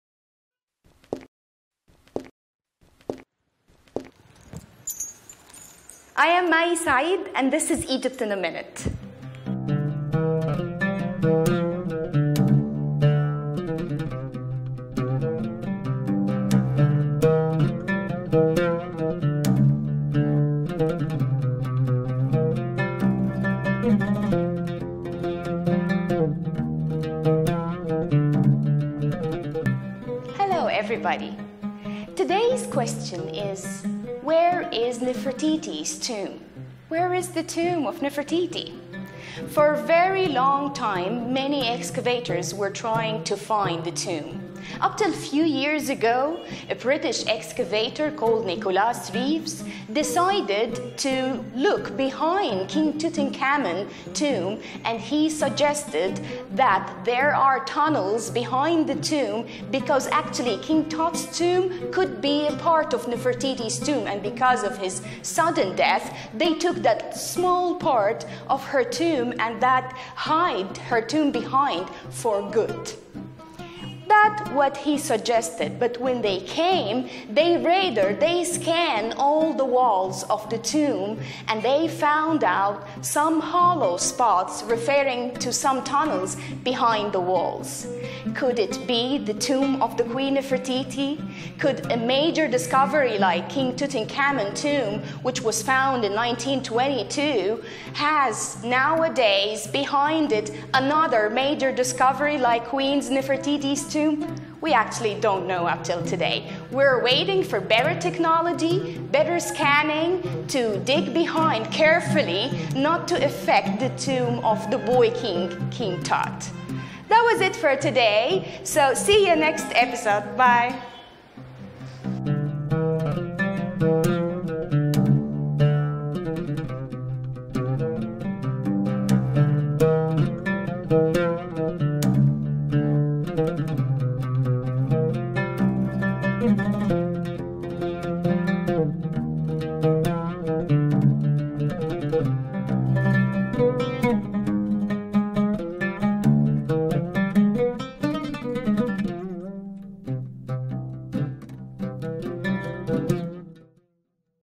Egypt in a Minute | Episode 4
Maii Saeed is an Egyptian blogger who wants you to see her country through her eyes. You can consider her as your time machine captin to Ancient Egypt and Modern Egypt too.

Egypt in a minute will be broadcasted exclusively on this YouTube channel every Saturday 10 am GMT

Digital Distribution by: ArpuPlus